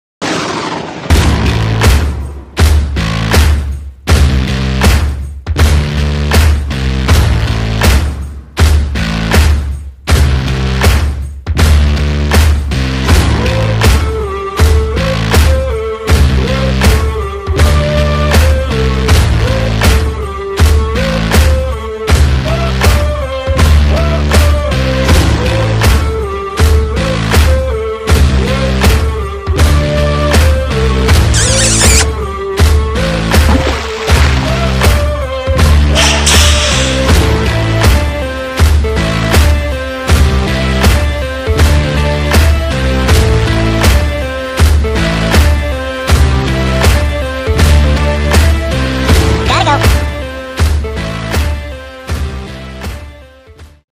Early Peconic Bay Fluke fishing
Quick clips of a long day with too many sea robins. Temps are finally starting to climb though!